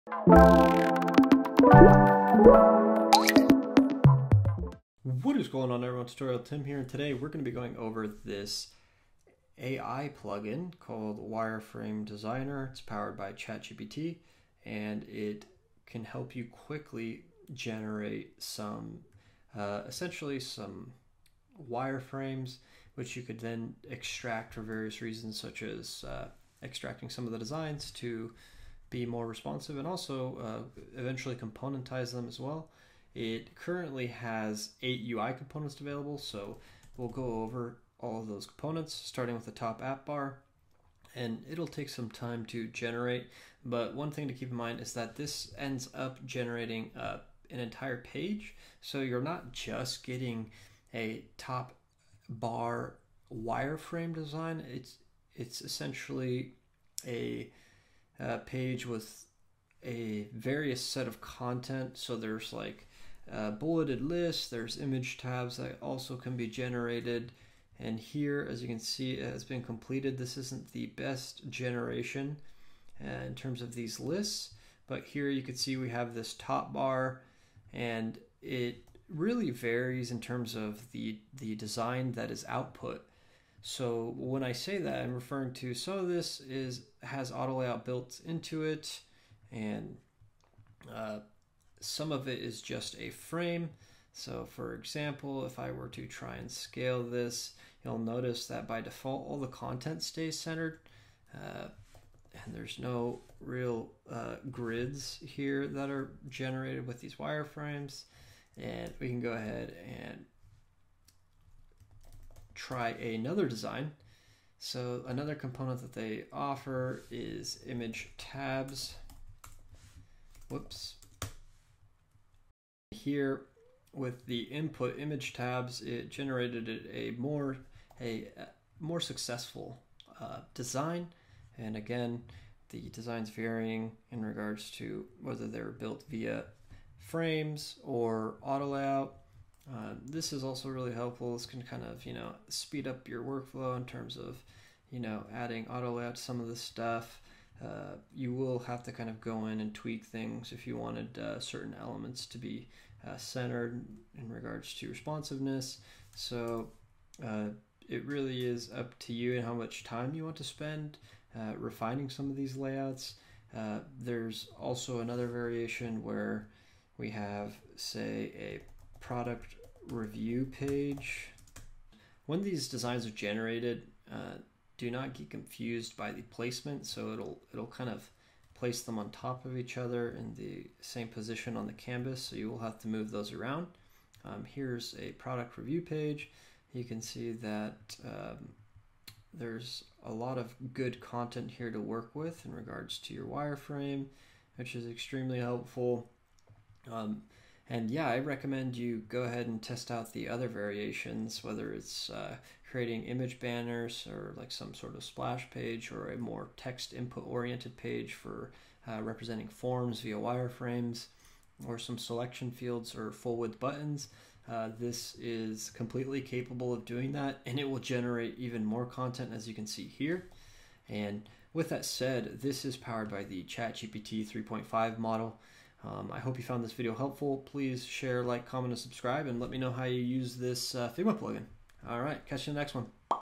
How to use the Wireframe Designer Figma Plugin
In this tutorial I show you how to use the ChatGPT powere Figma Plugin, Wireframe Designer.

Follow me on Figma!